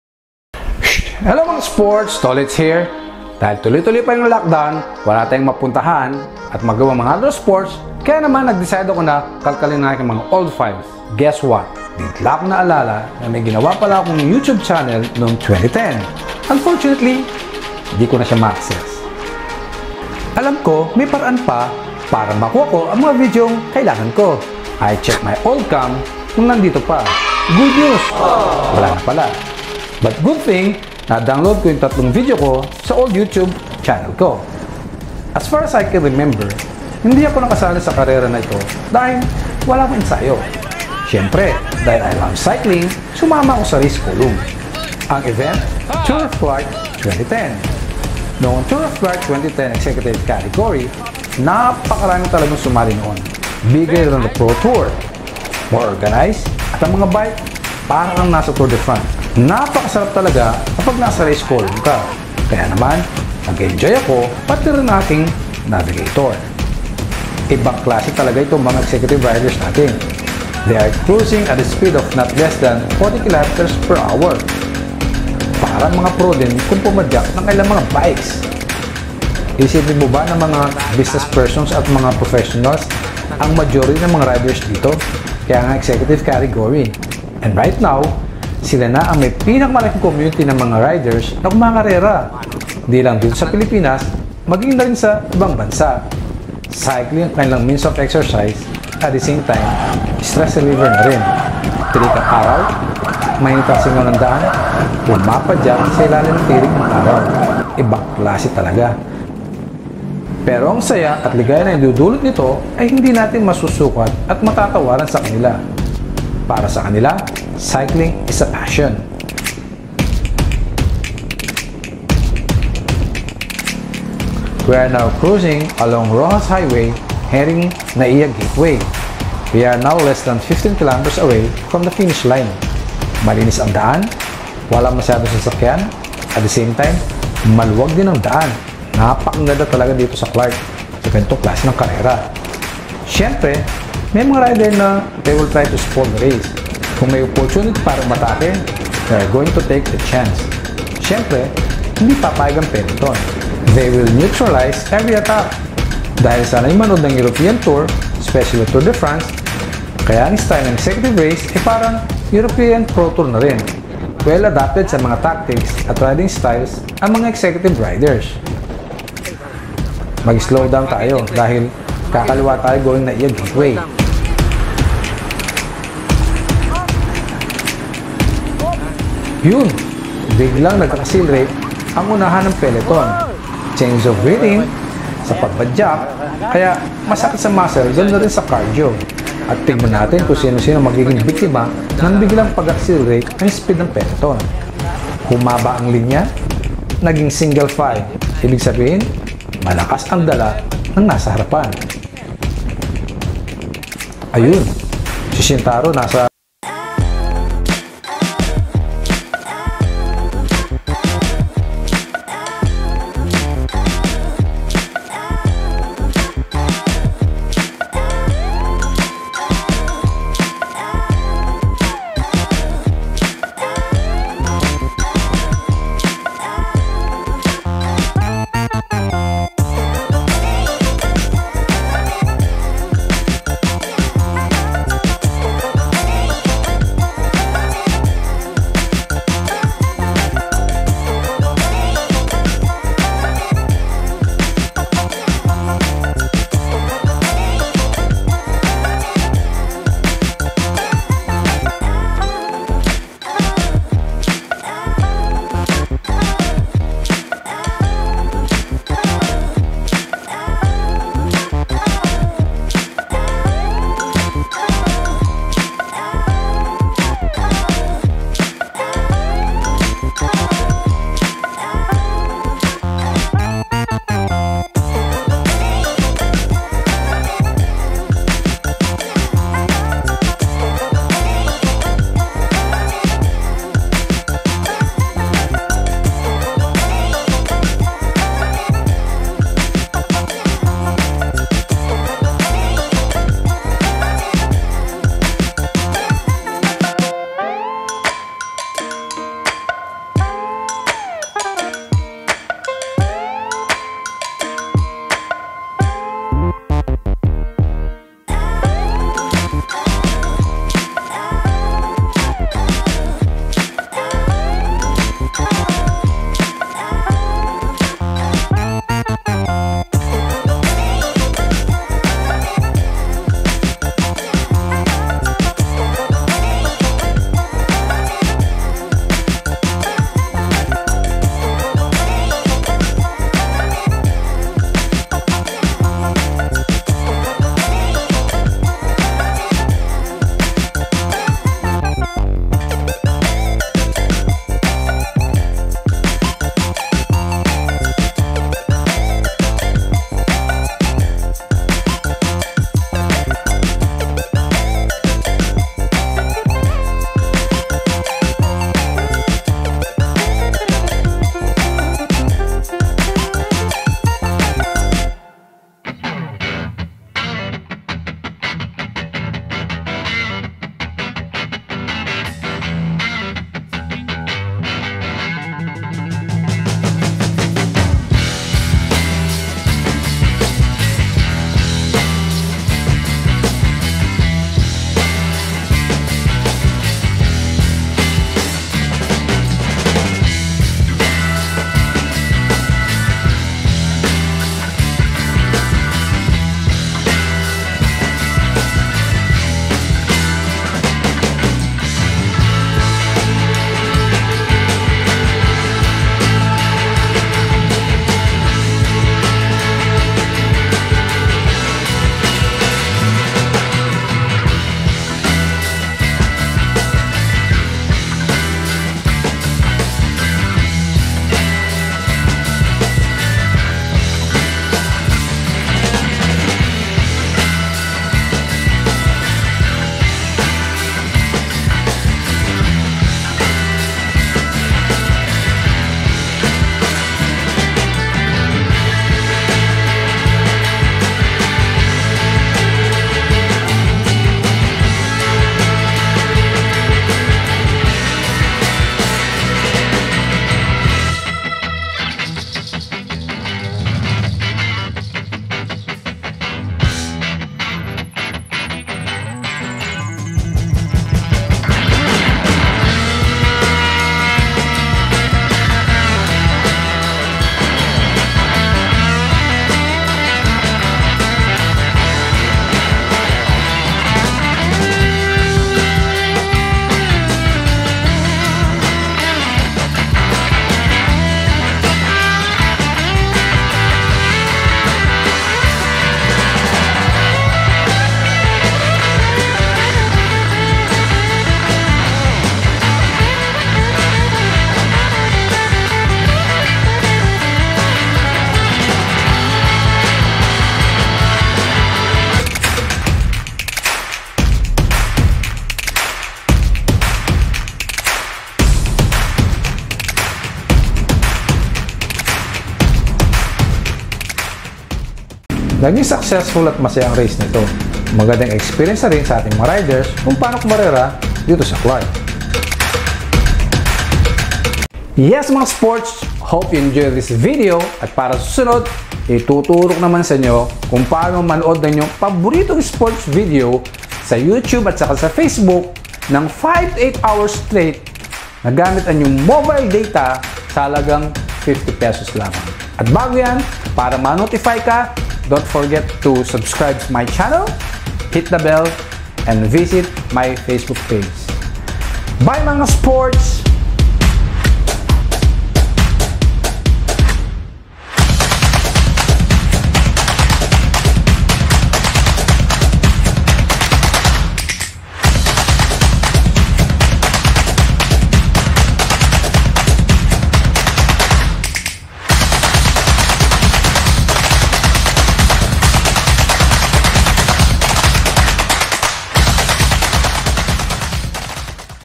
Enhance Community Quarantine | My Lock down Vlog Tour of Clark Executive Cycling Race
Dahil Lock Down ngayon. I searched my old file and found out that i created my first You Tube channel may back  2010. And again i would like to show  this video to refresh our memories esp during pandemic time
SHOUT OUT SA MGA RIDERS, Victor Mangahas Viray (Vec), Shintaro Valdes, Eric Buhain
at sa lahat ng mga executive Riders